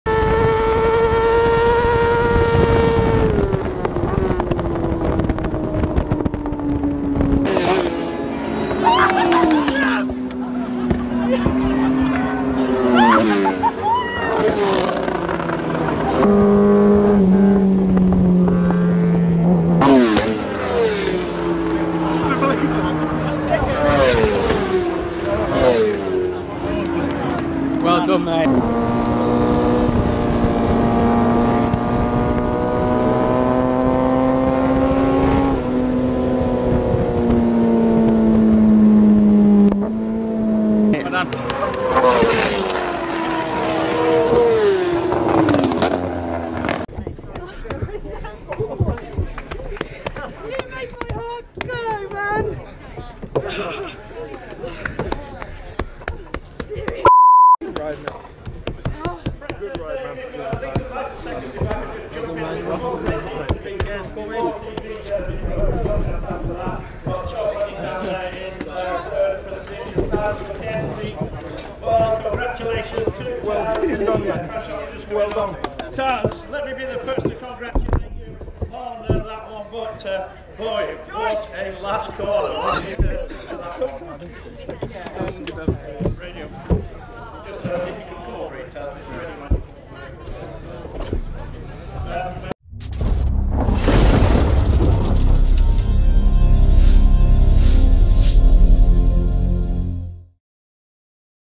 Josh Elliott brings home OMG's first ever Bennetts British Superbike victory
Check out the onboard and pit wall reaction to an incredible achievement in the history books of OMG Racing as Josh Elliott takes his and OMG's first ever victory in Round 1 of the 2019 Bennetts British Superbike Championship.

Competed for the first time - April 2018
First ever race win - April 2019

Onboard video courtesy of Action Elements and Bennetts British Superbikes